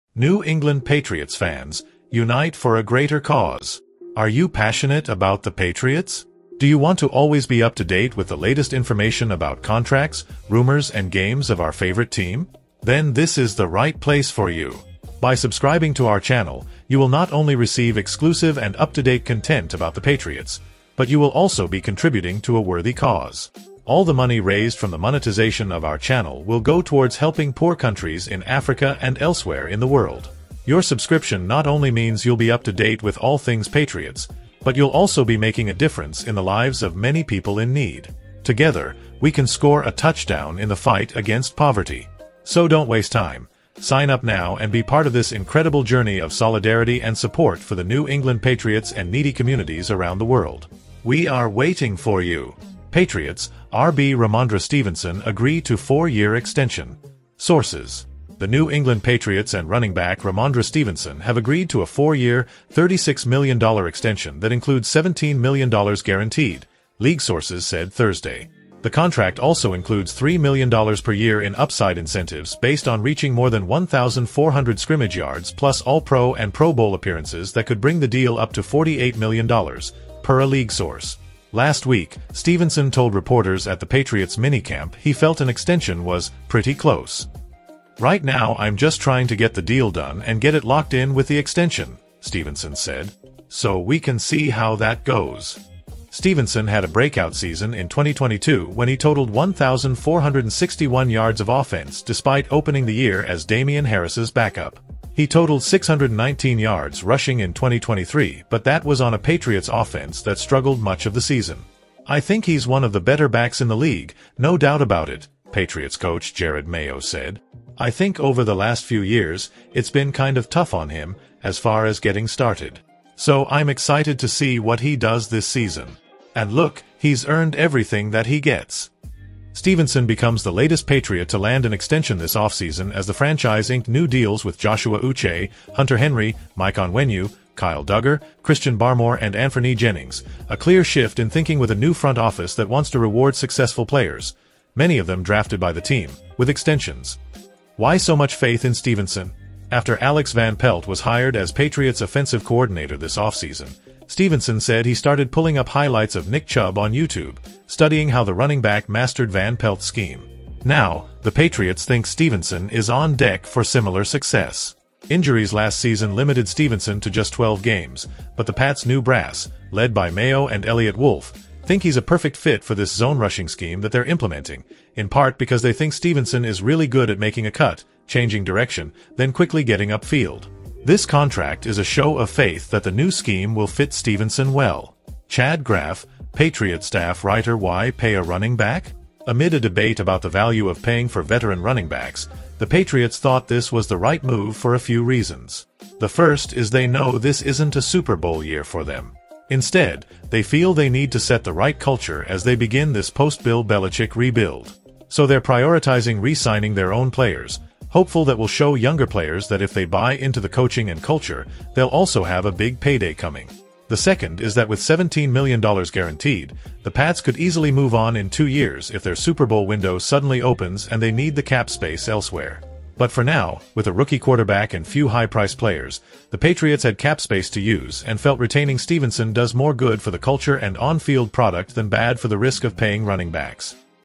BREAKING NEWS What Made the Patriots Pay Millions for Rhamondre Stevenson Find Out Now
What Made the Patriots Pay Millions for Rhamondre Stevenson Find Out Now!.

Patriots, RB Rhamondre Stevenson agree to 4-year extension: Sources
The New England Patriots and running back Rhamondre Stevenson have agreed to a four-year, $36 million extension that includes $17 million guaranteed, league sources said Thursday. The contract also includes $3 million per year in upside incentives based on reaching more than 1,400 scrimmage yards plus All-Pro and Pro Bowl appearances that could bring the deal up to $48 million, per a league source.
Last week, Stevenson told reporters at the Patriots’ minicamp he felt an extension was “pretty close.”

“Right now I’m just trying to get the deal done and get it locked in with the extension,” Stevenson said. “So we can see how that goes.”Stevenson had a breakout season in 2022 when he totaled 1,461 yards of offense despite opening the year as Damien Harris’ backup. He totaled 619 yards rushing in 2023 but that was on a Patriots offense that struggled much of the season.

“I think he’s one of the better backs in the league, no doubt about it,” Patriots coach Jerod Mayo said. “I think over the last few years, it’s been kind of tough on him, as far as getting started. So I’m excited to see what he does this season. And look, he’s earned everything that he gets.”

Stevenson becomes the latest Patriot to land an extension this offseason as the franchise inked new deals with Joshua Uche, Hunter Henry, Mike Onwenu, Kyle Dugger, Christian Barmore and Anfernee Jennings, a clear shift in thinking with a new front office that wants to reward successful players — many of them drafted by the team — with extensions.Why so much faith in Stevenson?
After Alex Van Pelt was hired as Patriots offensive coordinator this offseason, Stevenson said he started pulling up highlights of Nick Chubb on YouTube, studying how the running back mastered Van Pelt’s scheme. Now, the Patriots think Stevenson is on deck for similar success.

Injuries last season limited Stevenson to just 12 games, but the Pats’ new brass — led by Mayo and Eliot Wolf — think he’s a perfect fit for this zone-rushing scheme that they’re implementing, in part because they think Stevenson is really good at making a cut, changing direction, then quickly getting up field. This contract is a show of faith that the new scheme will fit Stevenson well. — Chad Graff, Patriots staff writerWhy pay a running back?
Amid a debate about the value of paying for veteran running backs, the Patriots thought this was the right move for a few reasons. The first is they know this isn’t a Super Bowl year for them. Instead, they feel they need to set the right culture as they begin this post-Bill Belichick rebuild. So they’re prioritizing re-signing their own players, hopeful that will show younger players that if they buy into the coaching and culture, they’ll also have a big payday coming.

The second is that with $17 million guaranteed, the Pats could easily move on in two years if their Super Bowl window suddenly opens and they need the cap space elsewhere. But for now, with a rookie quarterback and few high-priced players, the Patriots had cap space to use and felt retaining Stevenson does more good for the culture and on-field product than bad for the risk of paying running backs.